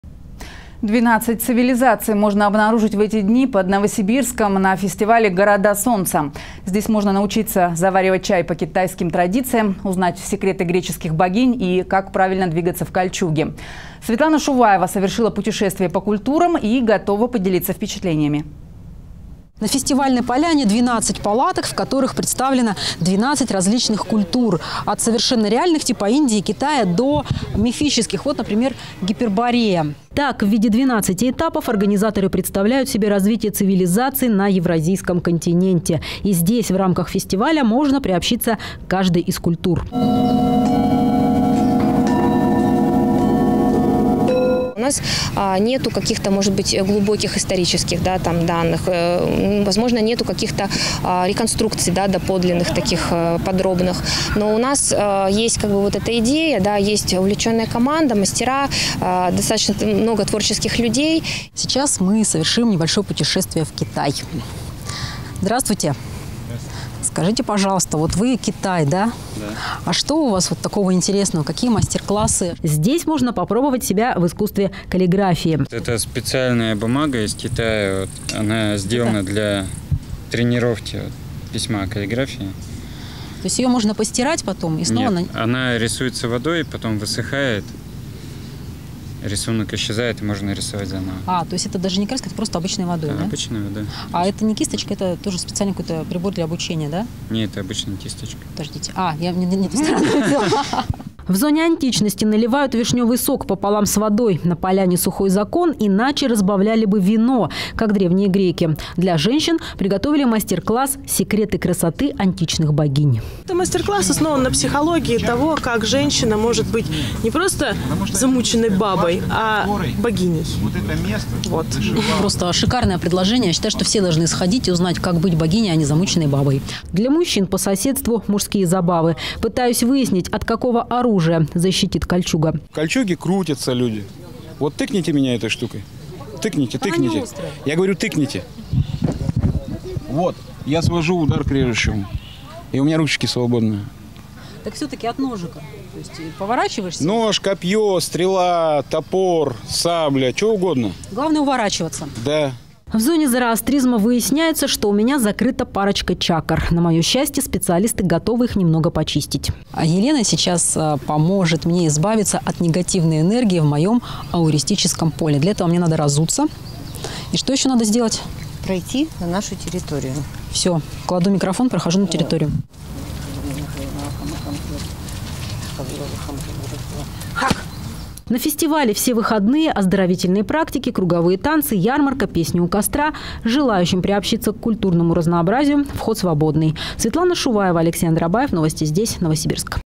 Фестиваль культур
12 цивилизаций под Новосибирском. Участники фестиваля культур ищут артефакты, чтобы исправить ошибки прошлого.